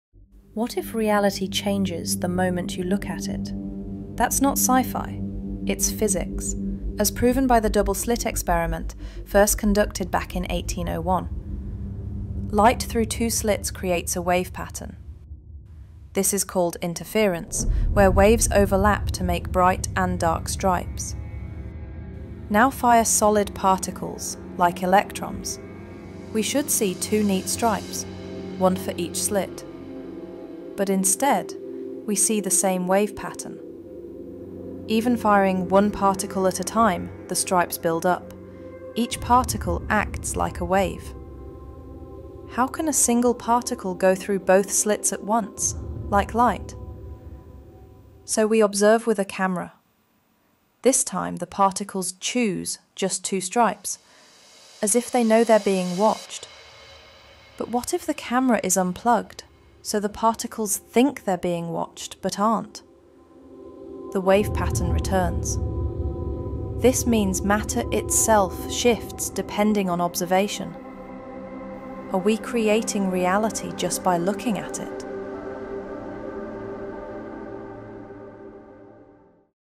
Observation Changes Reality. The Strangest Experiment in Science.
What if reality changes the moment you look at it? The double-slit experiment has puzzled scientists for over 200 years, and it might be proof that observation shapes the world itself.

In this short explainer, we break down Thomas Young’s legendary double-slit experiment (1801). The test that showed light acts like both waves and particles, and that electrons behave differently when observed. Some say it’s evidence of consciousness shaping reality.